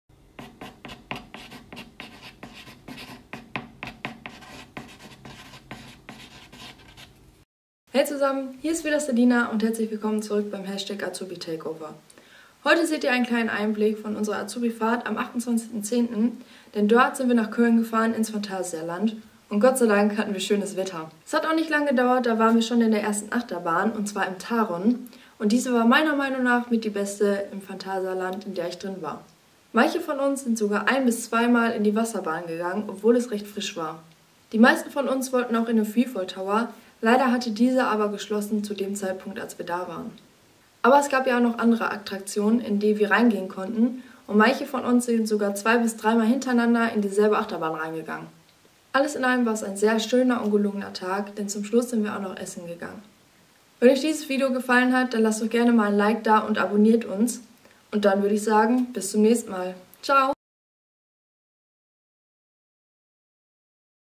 AzubiTakeover - Azubifahrt
Hey zusammen und herzlich Willkommen zurück beim Azubitakeover.🙋
Wir waren am 28.10 auf Azubifahrt im Phantasialand und haben einiges erlebt.
Dazu habe ich ein Video gemacht, in dem ihr ein paar Einblicke sehen könnt.🙂